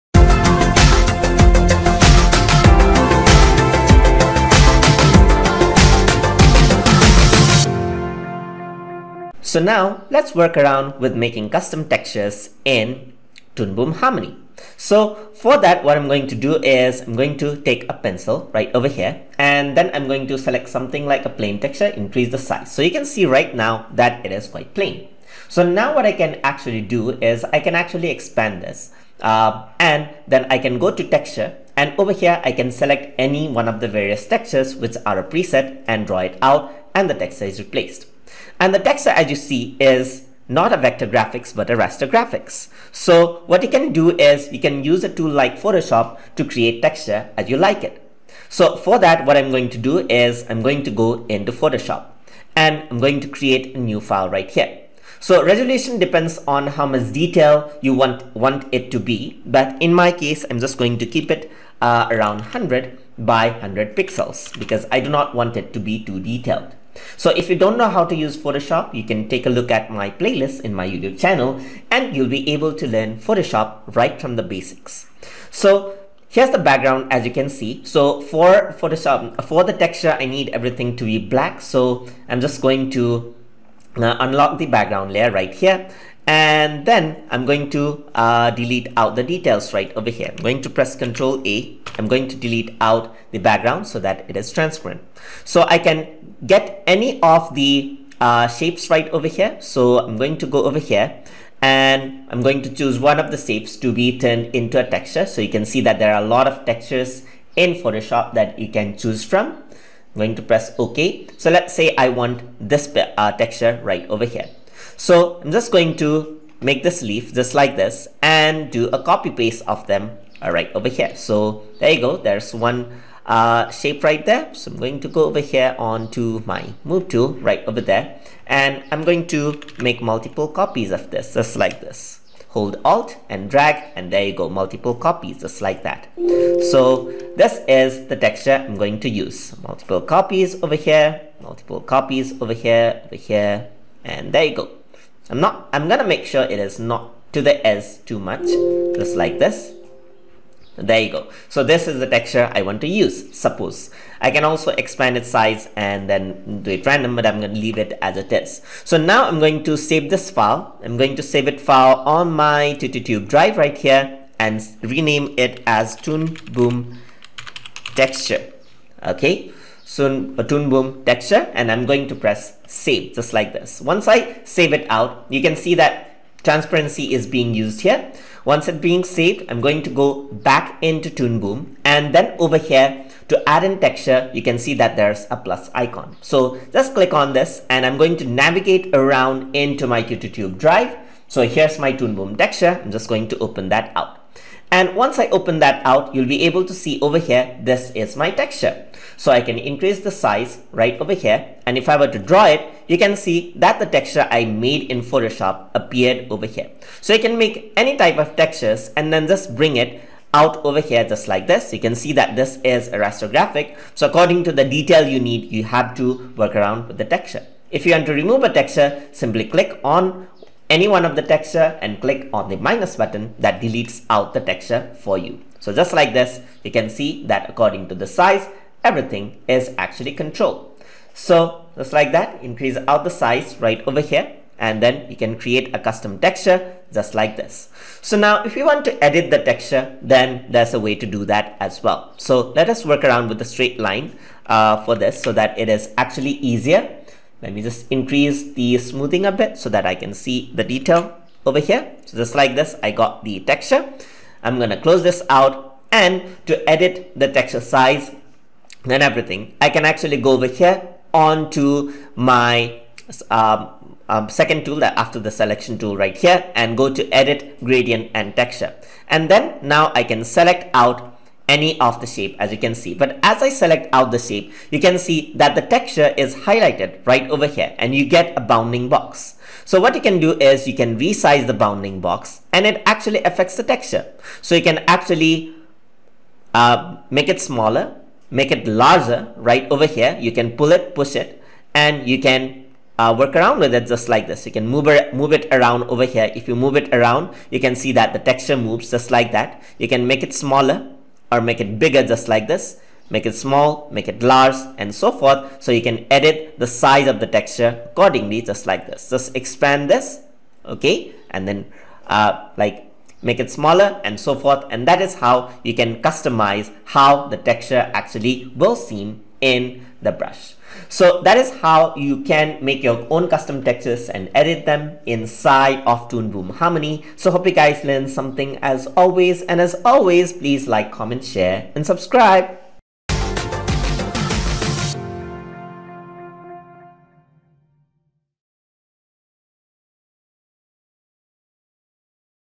Toon Boom Harmony Tutorial - Lesson 16 - Custom Pencil Texture and Texture Editing
In this tutorial, we will be discussing about Custom Pencil Texture and Texture Editing in Toon Boom Harmony.

Learn the basics of using Toon Boom including how to design, animate and produce 2D Animation.

This lesson has been made using Toon Boom Harmony 17 but is applicable for older and newer releases of the application as well.

If you like this video, here's our entire playlist of Toon Boom Harmony tutorials: